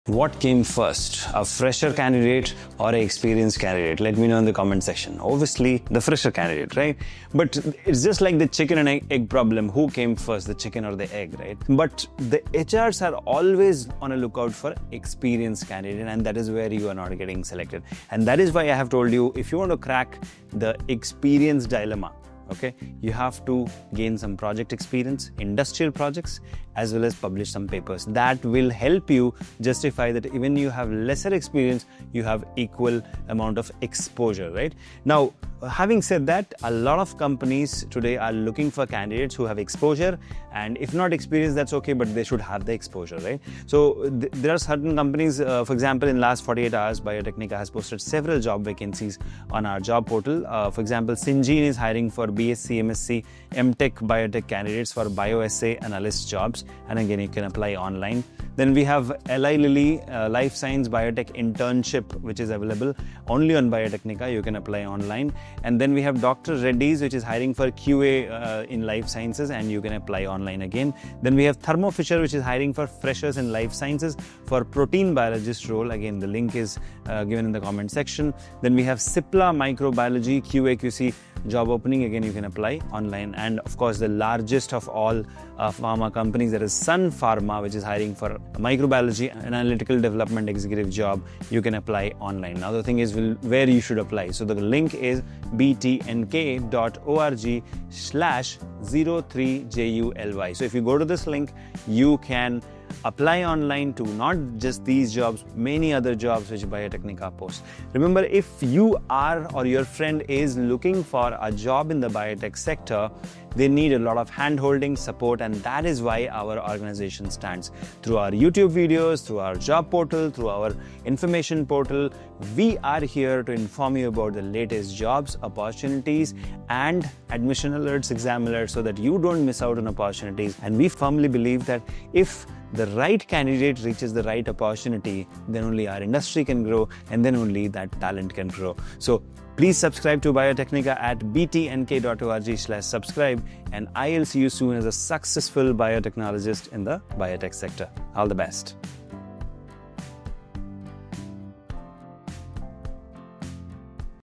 Lilly Internship, Freshers Jobs at Thermo Fisher, Syngene, Cipla, Dr Reddy's, Sun Pharma & More
A tricky question, right? But it’s one every biotech or life science aspirant faces!
In today’s video, we’ll break down the classic career dilemma –
Do companies want experience first, or are freshers still in demand?

🎯 Struggling to get your first job?
🎯 Wondering if internships or hands-on training will help?
✅ Learn the smart way to position yourself in the biotech industry – whether you're a fresher or have some experience.
✅ Real-world tips for students and job seekers in Biotechnology, Life Sciences & Bioinformatics.

🎓 Biotecnika – Empowering Every Scientist’s Dream